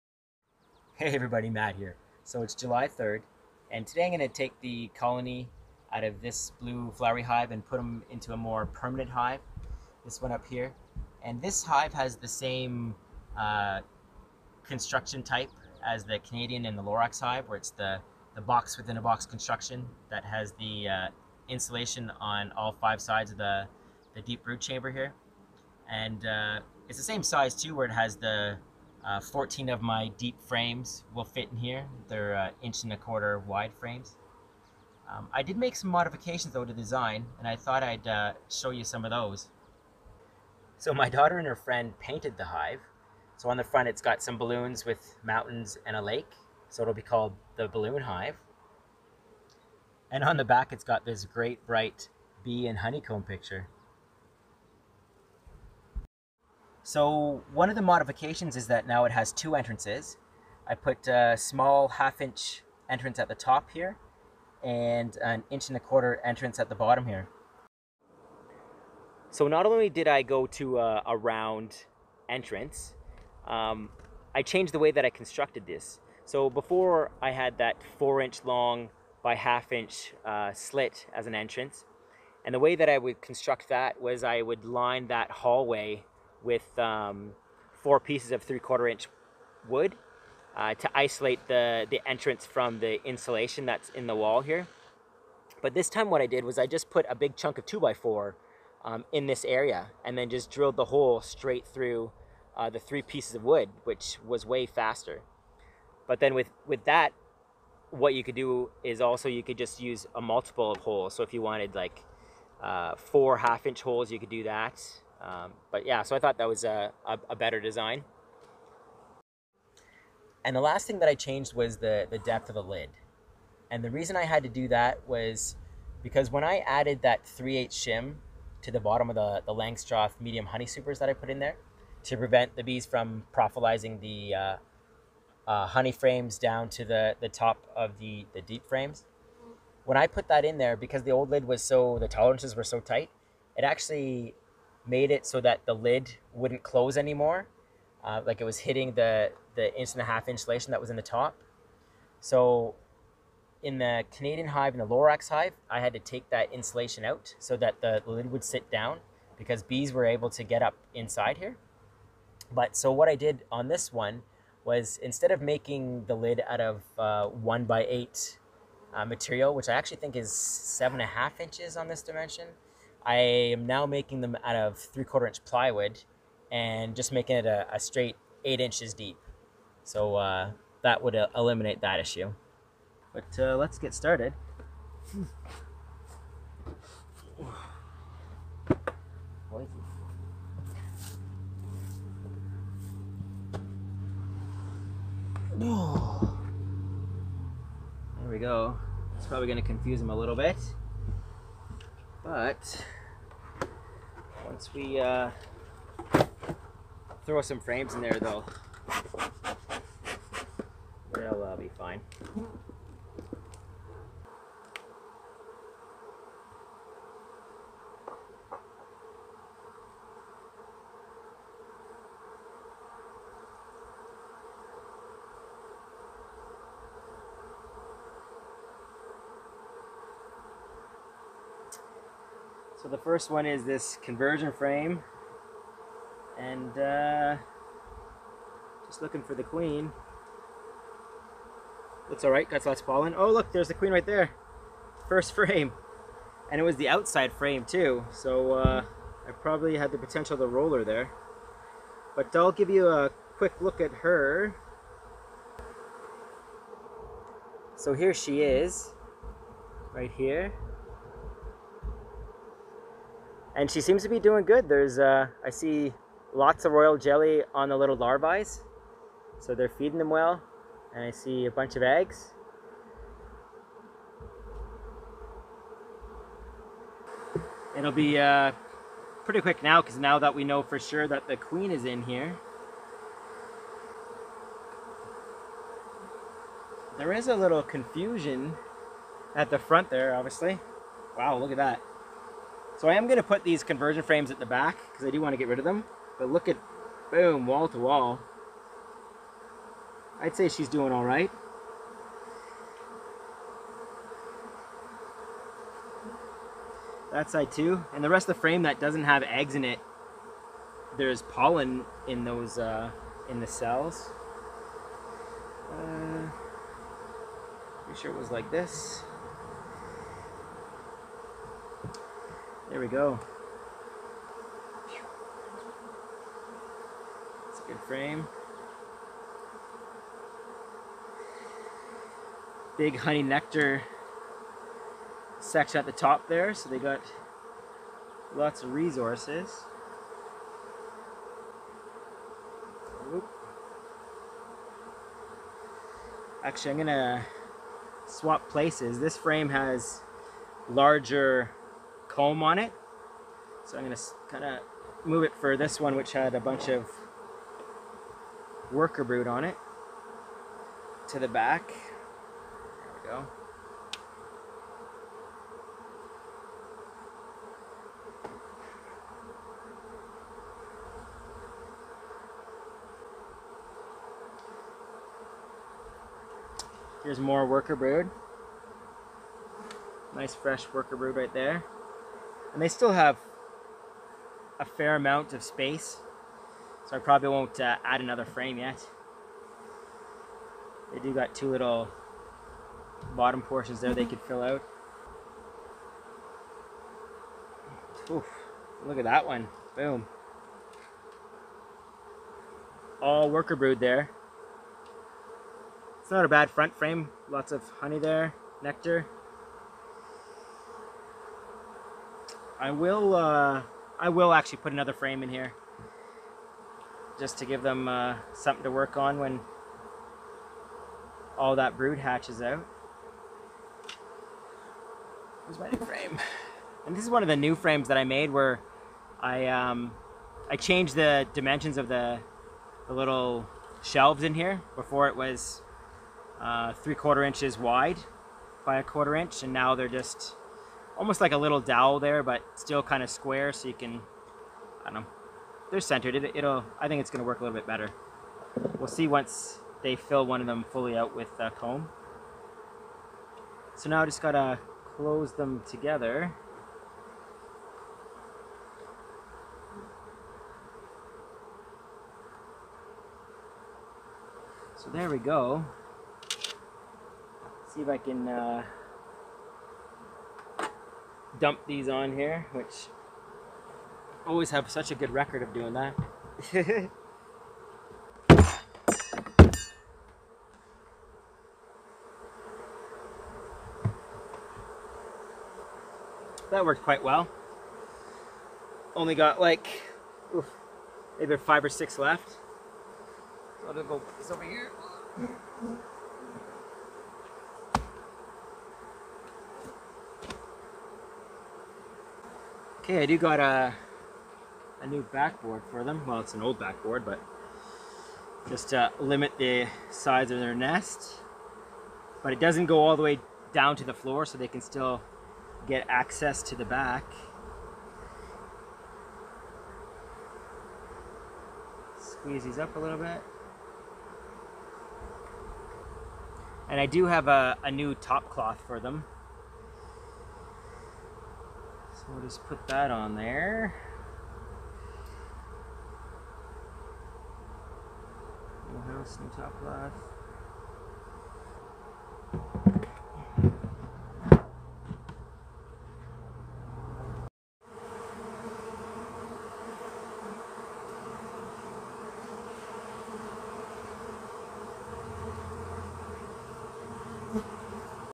July 3rd 2019 - Hive Design Changes - Matt's Vlog #154
Today I moved our  May 11th swarm colony into one of my new 14 frame deep hives. I thought I would show you a few modifications I made to my design.